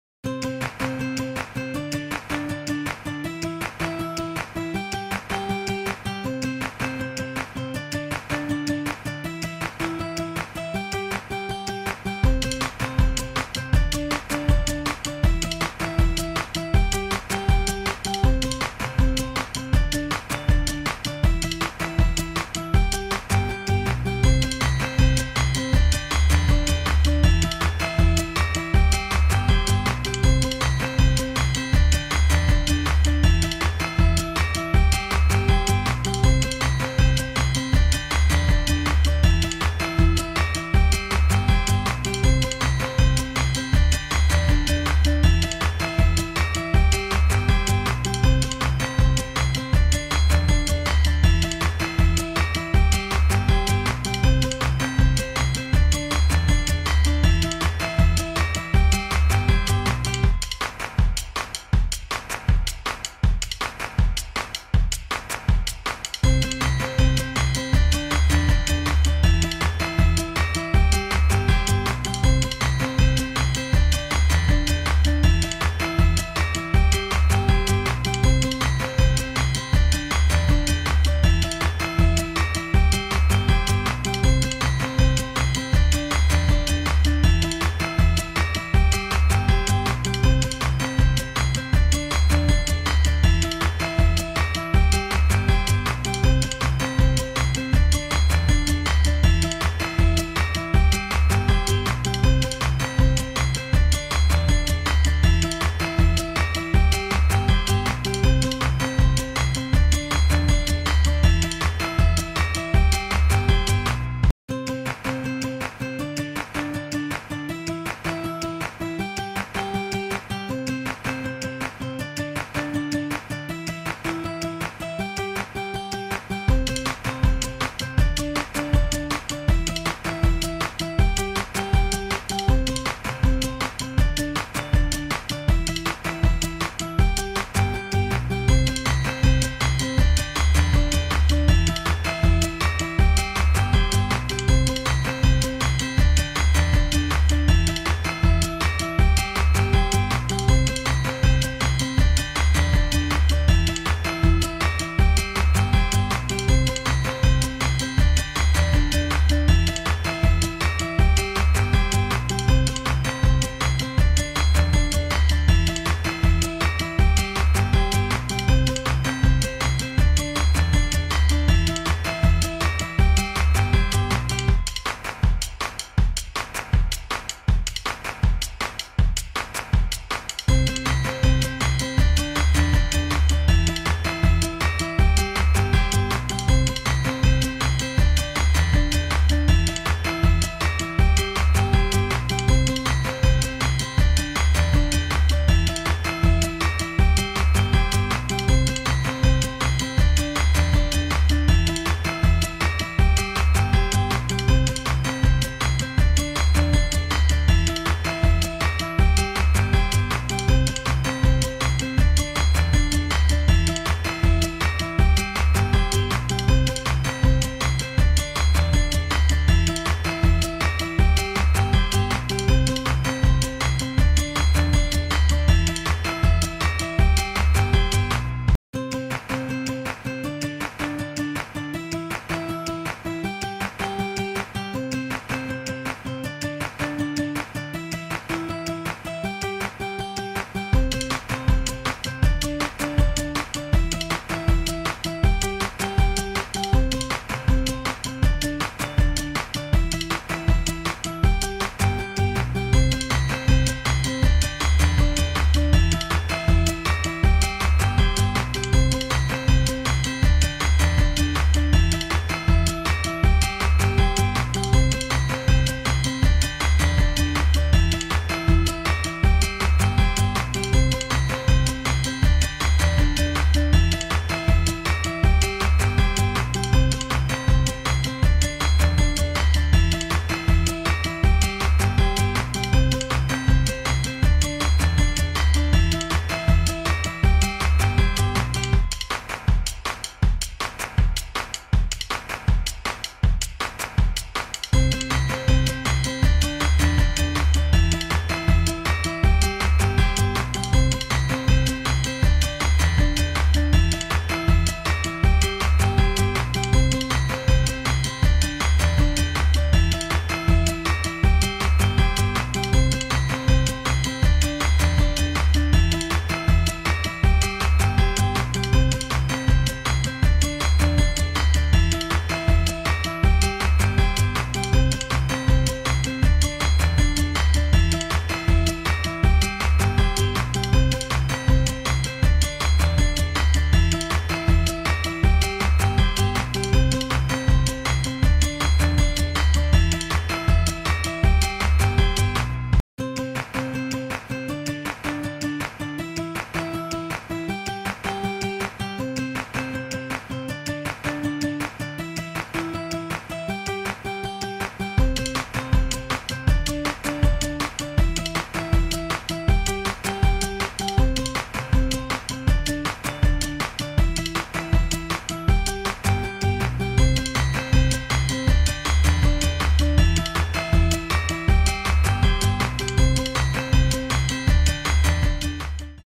FROZEN ❄️ linda decoracion, torta y bocaditos personalizados 💎frozen decoration
Frozen, decoracion temática infantil, descubre como hacemos  esta linda decoracion de fiesta para cumpleaños de niña de frozen una aventura congelada con torta deshielo y bocaditos personalizados, Elsa, Ana, Olaf, kristoff, diy party decoration, frozen decoration.